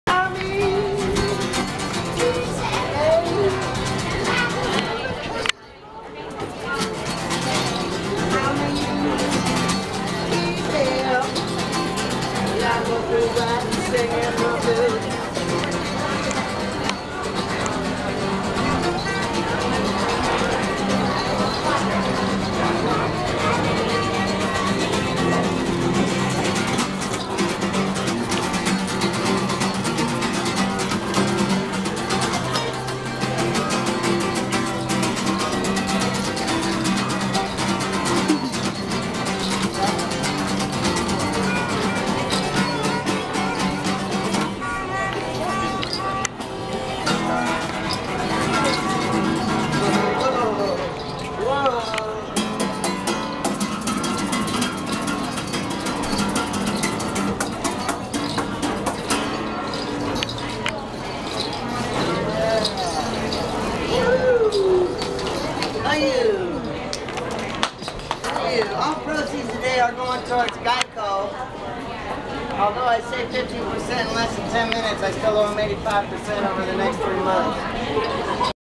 Hula-hoopin', Guitar & Harmonica-playin' dude in Seattle
Pikes Market